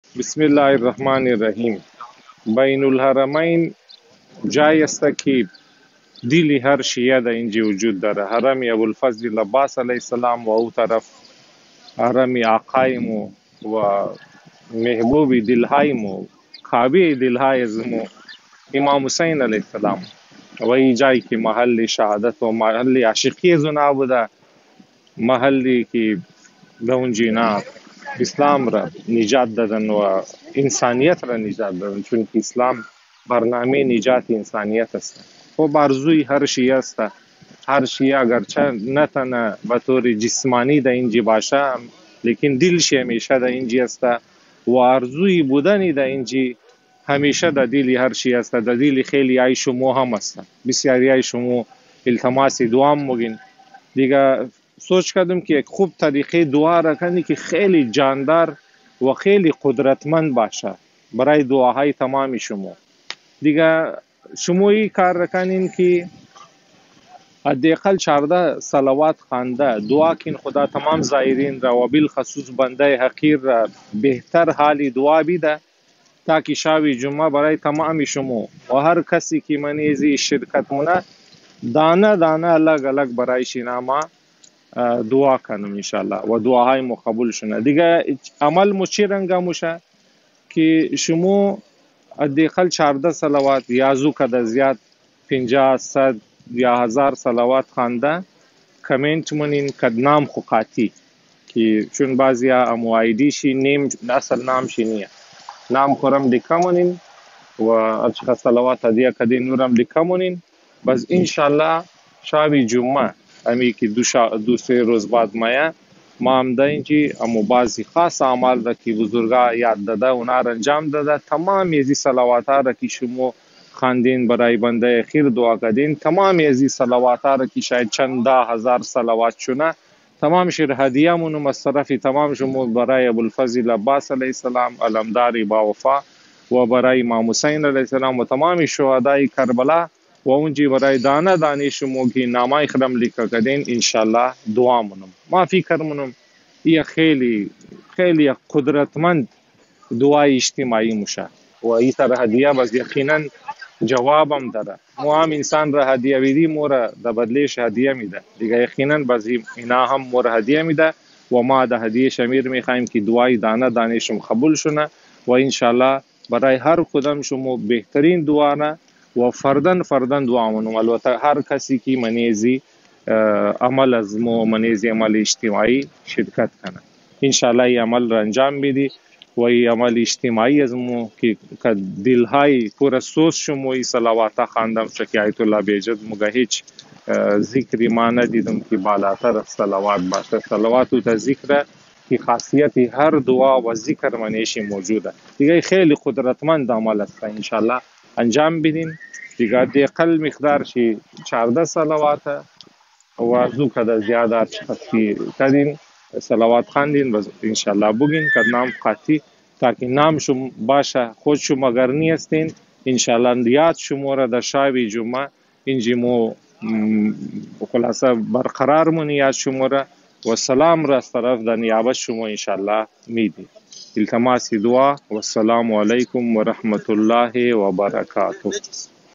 Karbala Friday Night's Dua - قدرتمند دعای اجتماعی شب جمعہ در کربلا
شرکت کین در قدرتمند دعای اجتماعی شب جمعہ در کربلا
هدیہ هزاران صلواتِ اجتماعی برای قبولیت دعاهای شمو
Strong Dua in Bain ul Haramain for You All.
نائب الزیارت هم استوم ان شاء الله

از طرف: خواهران منجی
خدا تمام ازی خواهران را نگاه کنہ